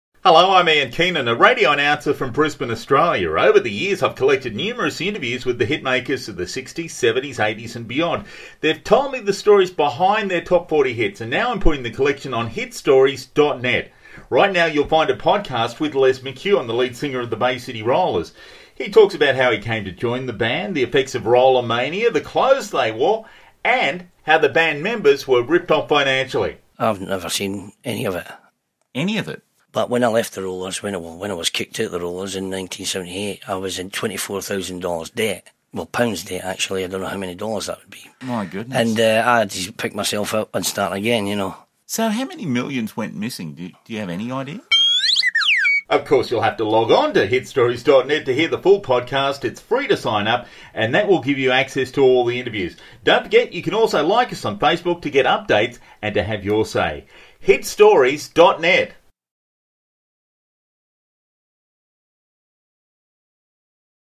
Bay City Rollers' lead singer, Les McKeown, on Hitstories.net
to access hundreds of interviews with musicians including Les McKeown, the lead singer of the Bay City Rollers. Hear the stories behind the hits!